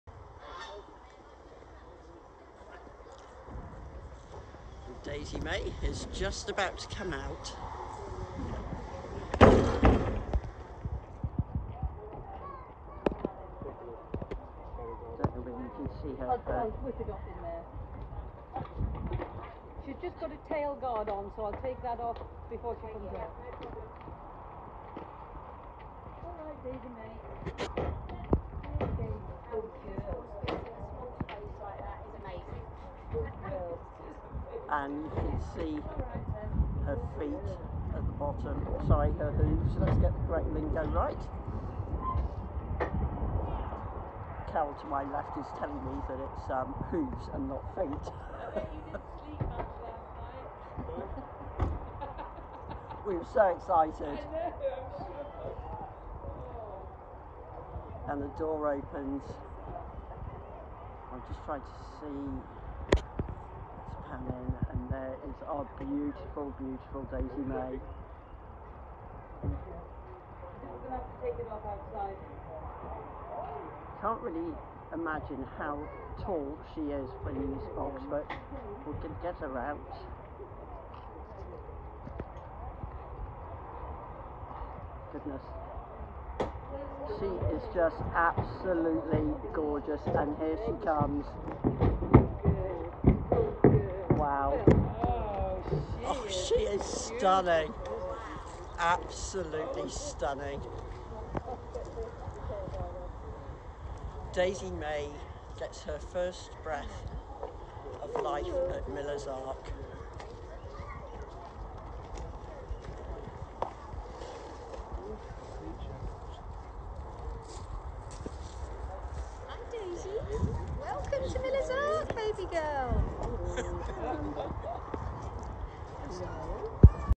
Daisy Mae arrives at the farm
Daisy Mae our latest addition to join the Miller’s Ark family arrives on the farm.